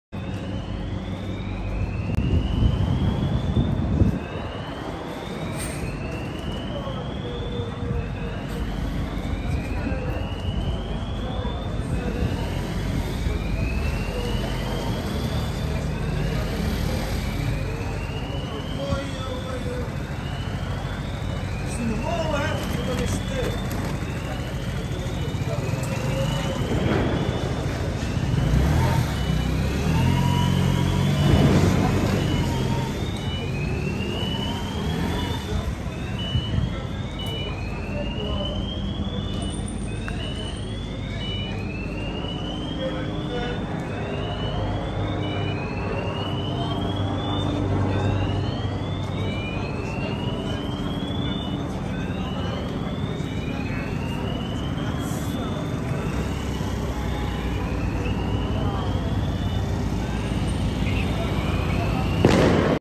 Verlaten politie-auto's na rellen in Rotterdam vandaag
ROTTERDAM - Verlaten politie-auto's na rellen in Rotterdam vandaag.

Sluw Nieuws NL 2021🇳🇱